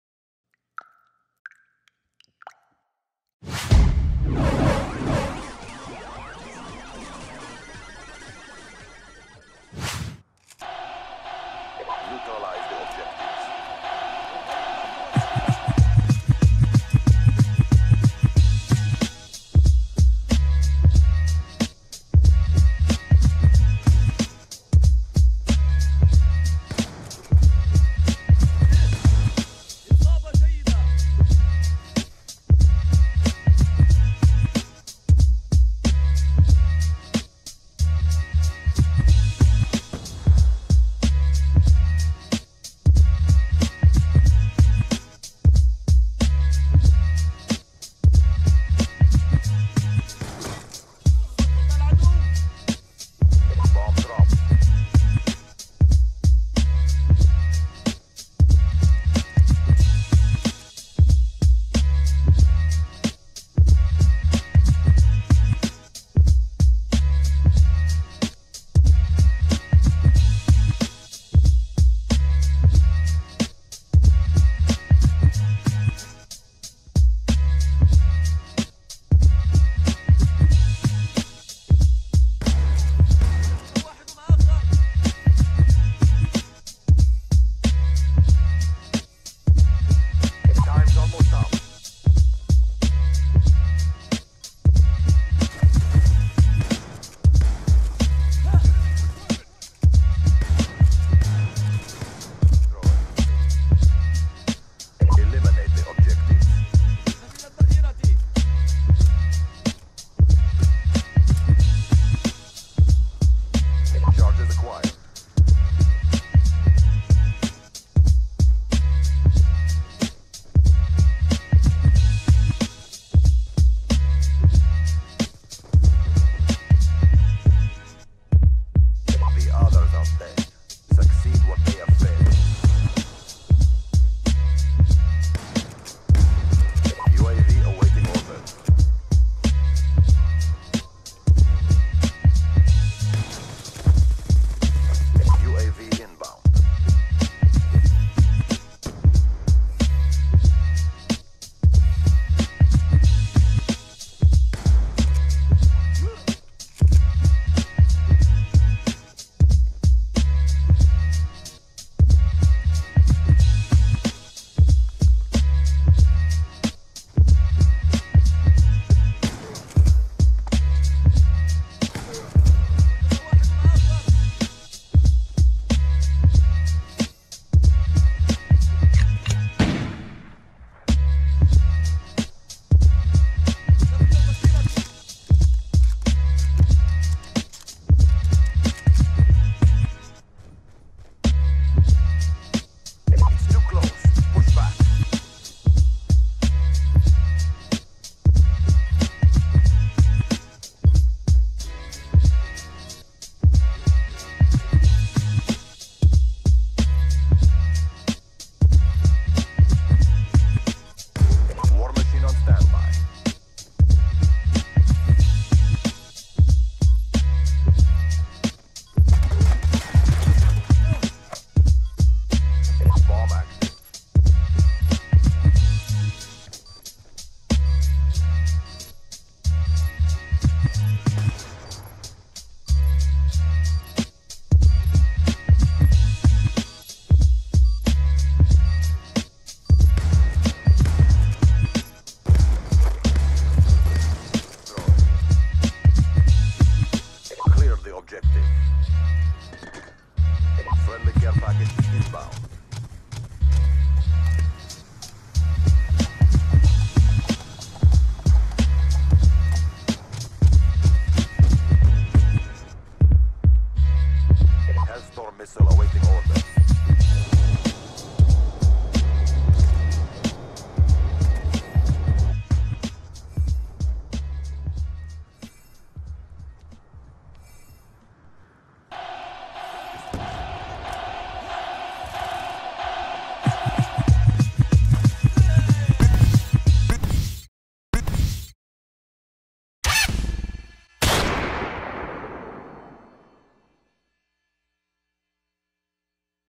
Best S&D Defeat Ever 1v6 | 13-1 W/ KSG | Turbine | Trvsblackwell
Joined late at 0 to 3. Only 3 people on my team against 6. Sweet 1v6 clutch in round 5.  Please leave me a comment :)